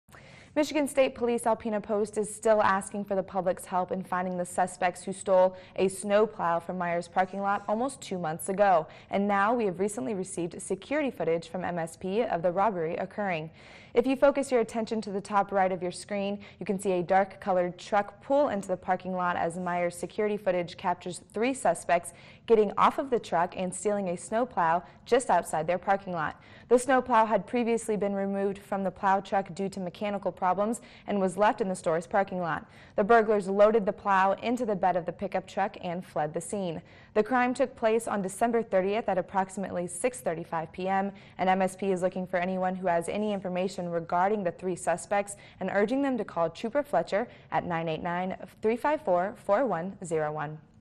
MSP Releases Security Footage of Stolen Plow
Michigan State Police Alpena Post is still asking for the public's help in finding the suspects who stole a snow plow from Meijer's parking lot almost two months ago, and now, we have recently received security footage from MSP of the robbery occurring.